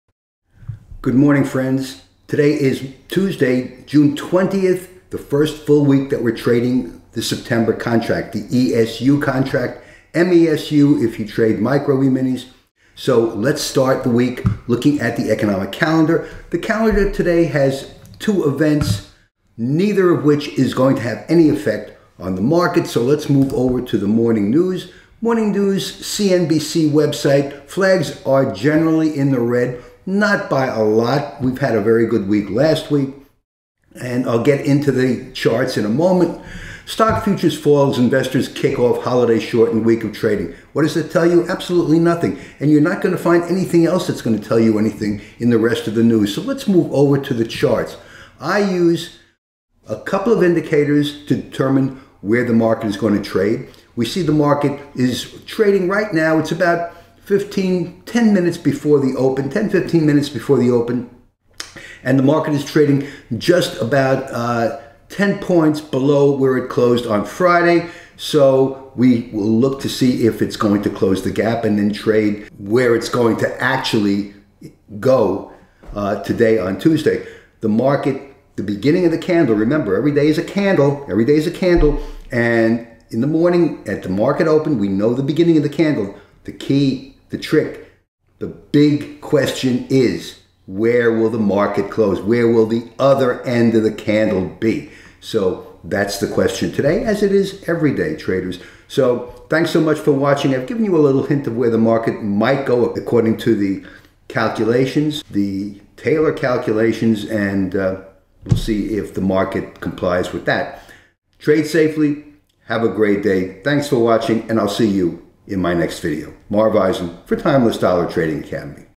Where Will Eminis Trade After Juneteenth
Where will the market trade following the Juneteenth holiday? That's the question today, traders. But that's the question every day.
This video gives a hint in answering this question.